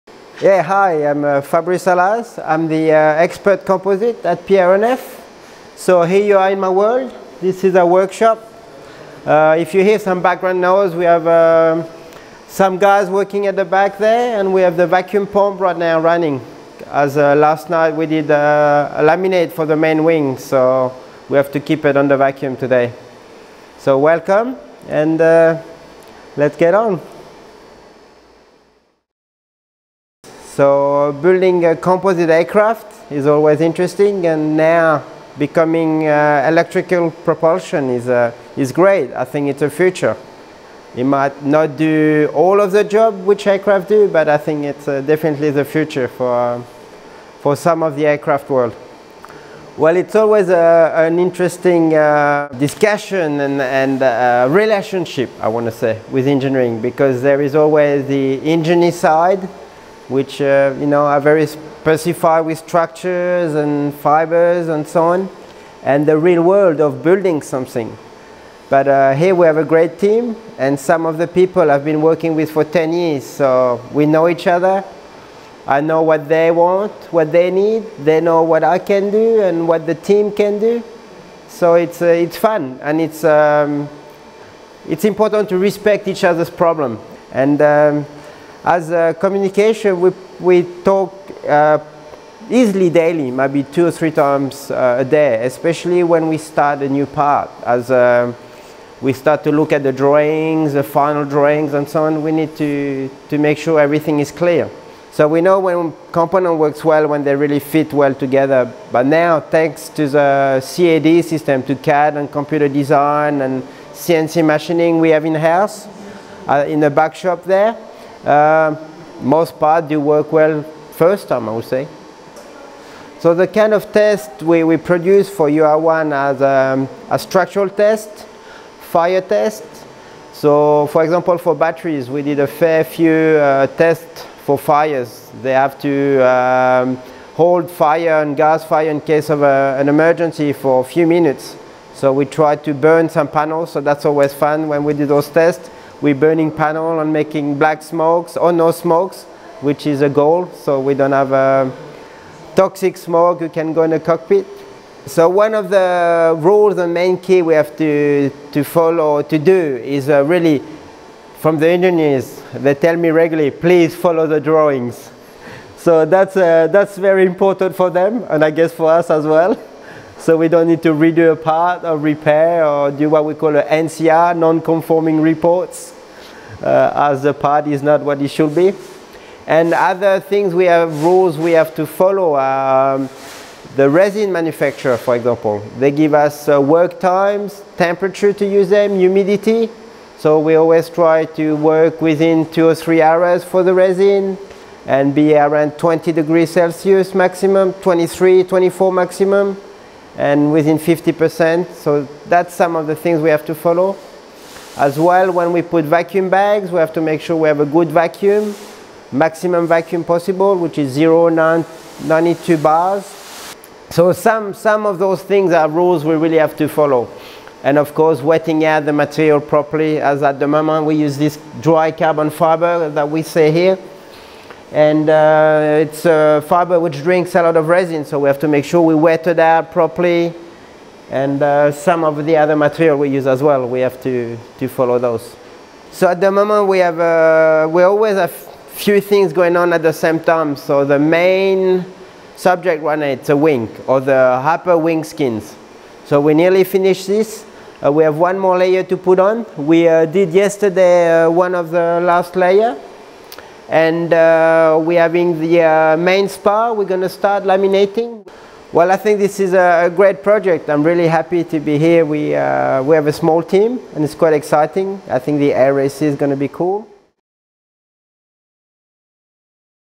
Manufacturing aircraft parts in composite | Interview with Fabrice Allaz
In this interview, Fabrice Allaz, Expert in composite materials at Pie Aeronefs SA, explains you the processes and some information about the steps required in order to manufacture parts in composite for the first Swiss all-electric race plane.